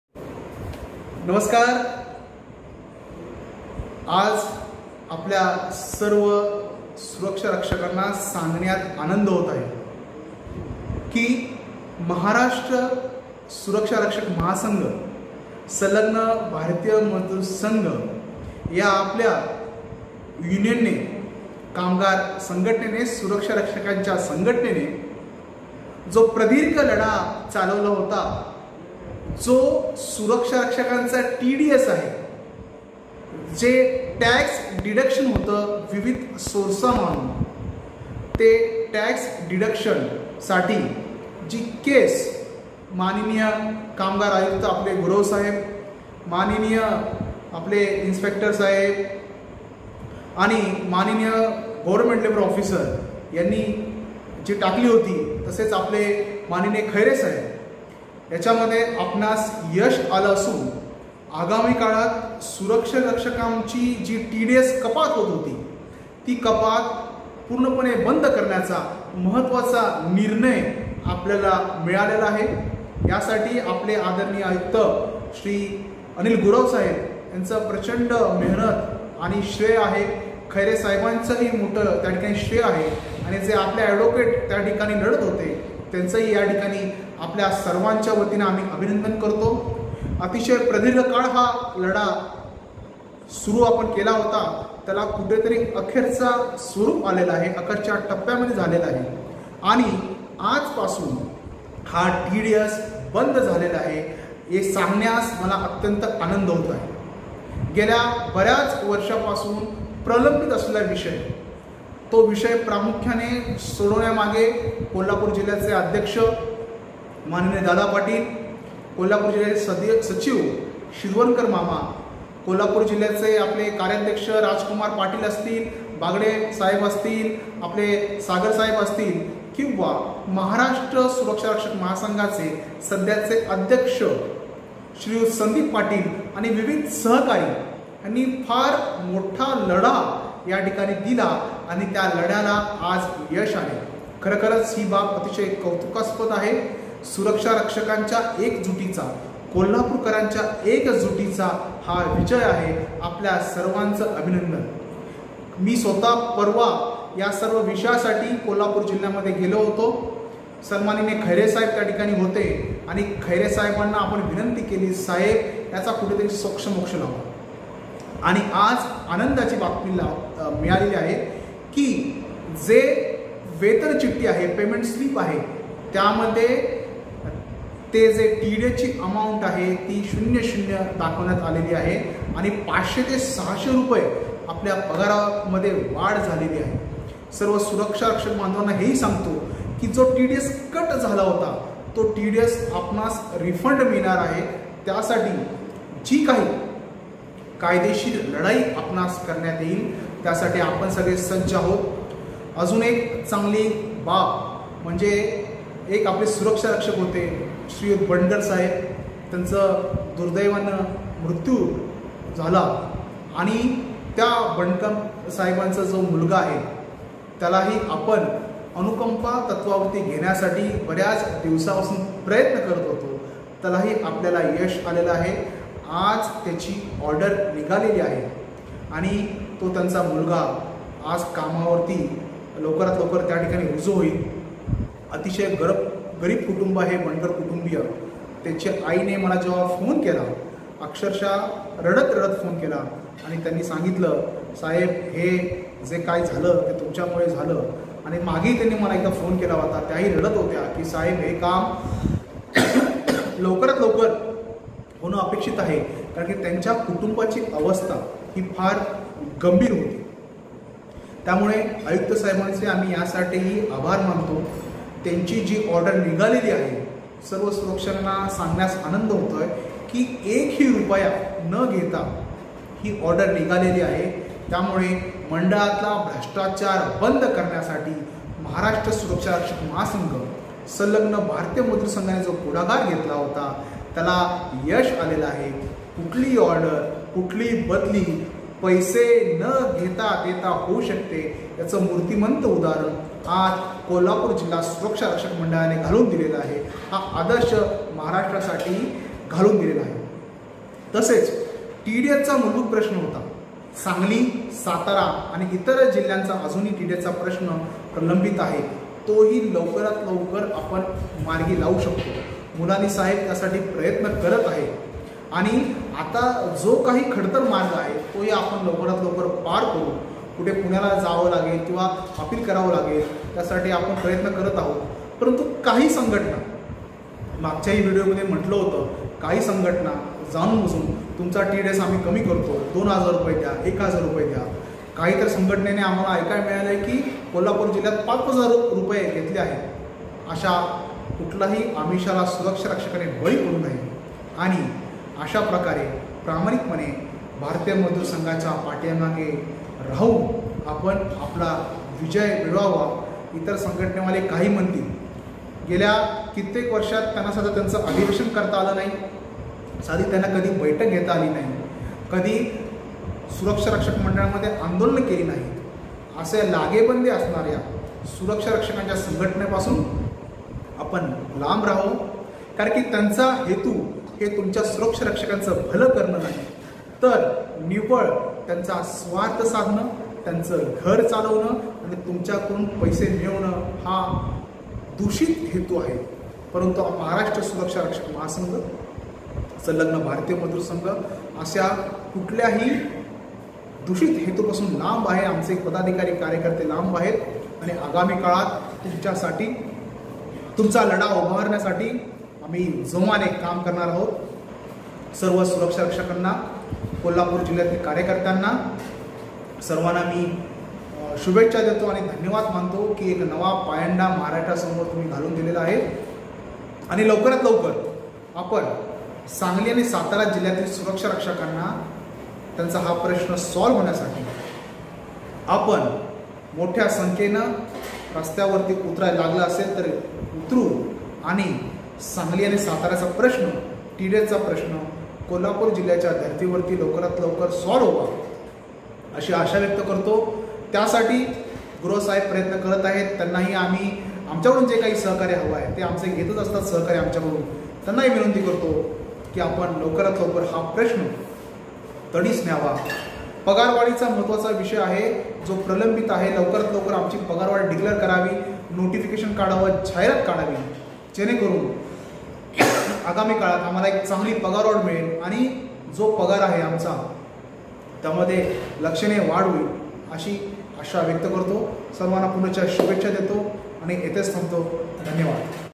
TDS बंद 😀
कोल्हापूर जिल्हा सुरक्षा रक्षक मंडळाचे अभिनंदन सुरक्षारक्षकांचा टीडीएस बंद केले बाबत🌹